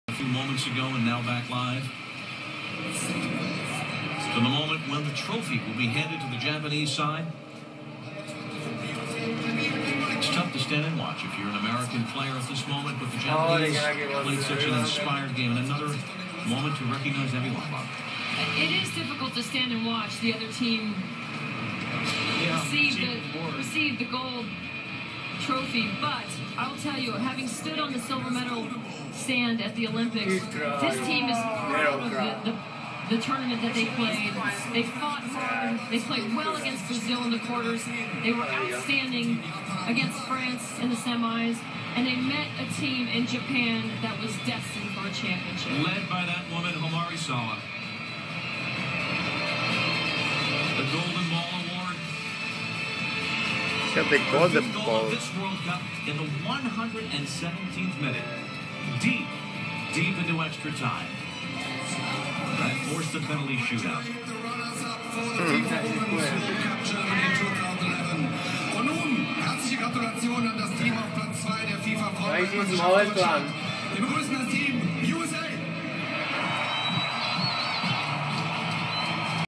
Trophies/Awards for Solo, Wanbach and Sawa for Best Players in Women's WC
Best players of the Women's World Cup Germany 2011 Hope Solo, Aby wanbach, Homare Sawa, but Japan took it All.